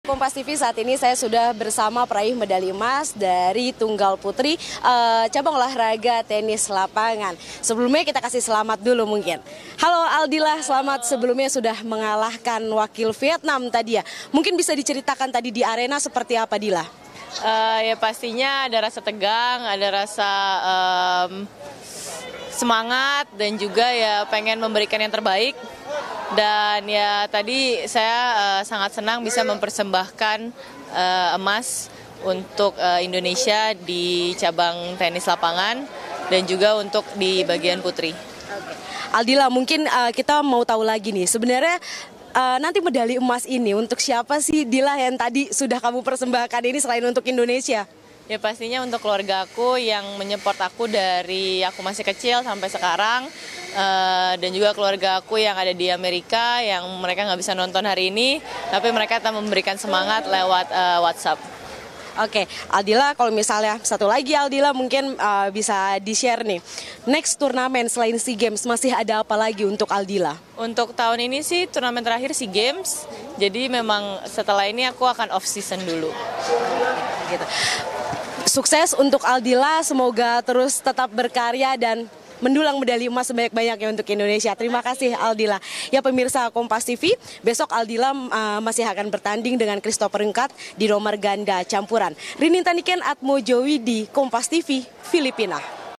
Eksklusif! Aldila Sutjiadi,Peraih Emas Tunggal Putri Tenis Lapangan Usai Sewindu Puasa Gelar
Petenis indonesia Aldila Sutjiadi berhasil meraih medali emas tunggal putri pada Sea Games 2019 Filipina. Medali emas tunggal putri ini merupakan yang pertama setelah pencapaian Ayu Fani Darmayanti pada Sea Games 2011 di Palembang, Indonesia.

Kompas Sport memiliki wawancara eksklusif Aldila Sutjiadi bersama Jurnalis KompasTV Rinintha Niken dan juru kamera Atmojo Widi.